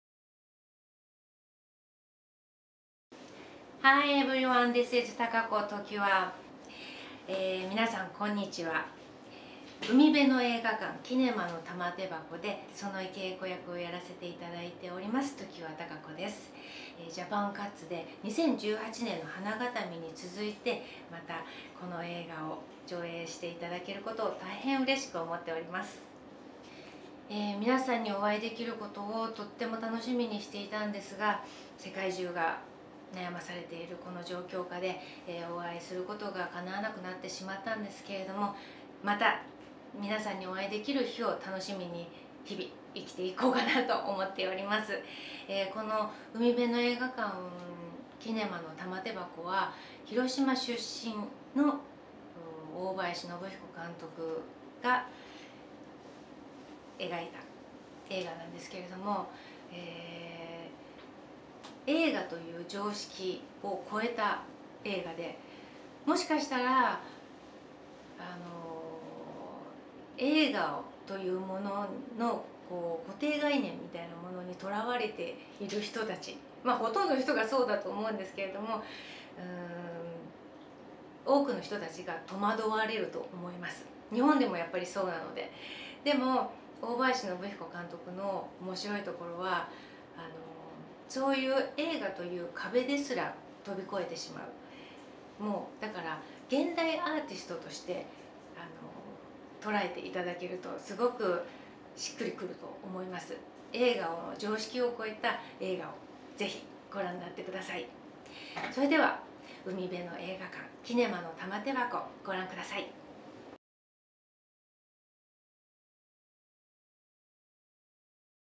Takako Tokiwa - JAPAN CUTS 2020
Star, Labyrinth of Cinema

Takako Tokiwa, born 1972, is an actress who made her sensational debut in Fuji Television's TV Series Devil's Kiss in 1993. Bringing presence and spirit, she started her career as an actress and continually appeared as lead characters and heroines since. Her unforgettable performances include TBS’s Tell Me That You Love Me, and also TBS’s blockbuster series Beautiful Life. She also starred in several films directed by Nobuhiko Obayashi, including Seven Weeks (JAPAN CUTS 2015), Hanagatami (JAPAN CUTS 2018), and Labyrinth of Cinema (2019).